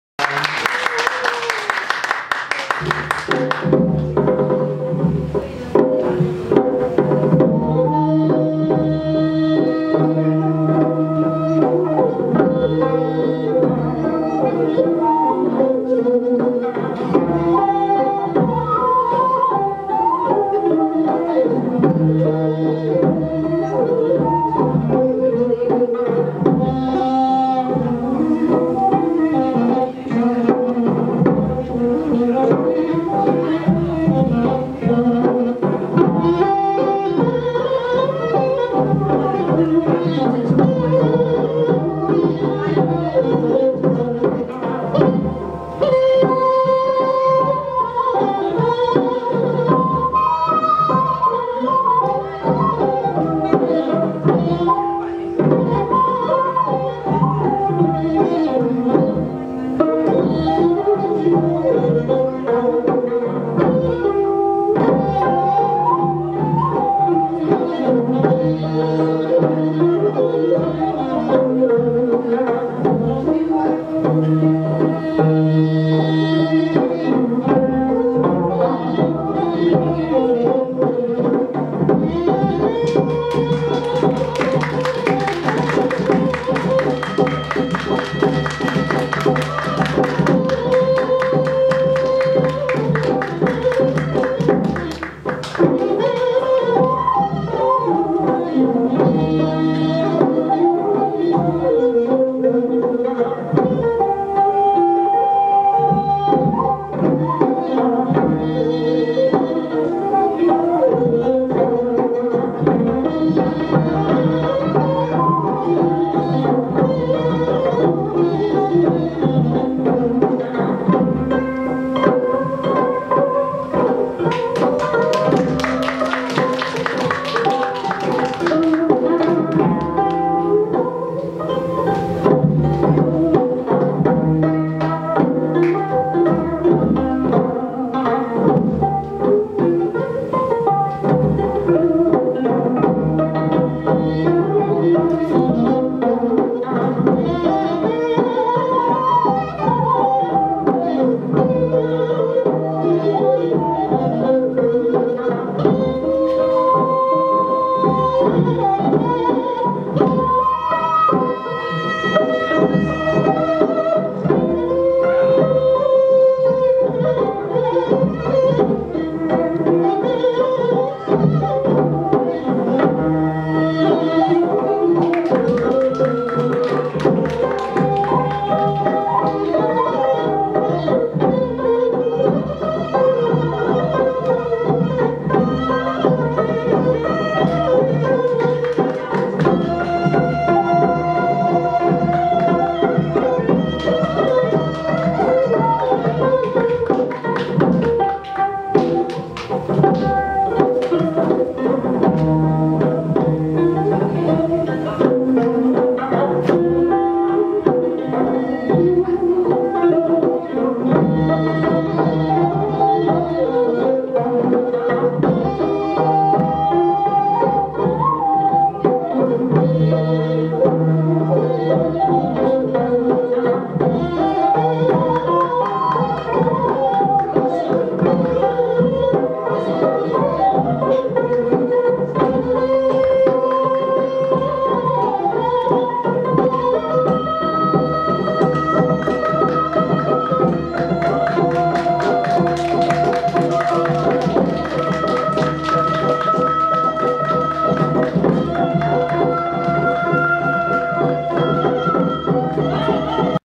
초대 김예지 한국 고전무용 제2회 트로트 가요세상 콘서트 2017년 1월 15일 수원라이브 7080 하태춘
알콩달콩 예술단 촬영감독 하일호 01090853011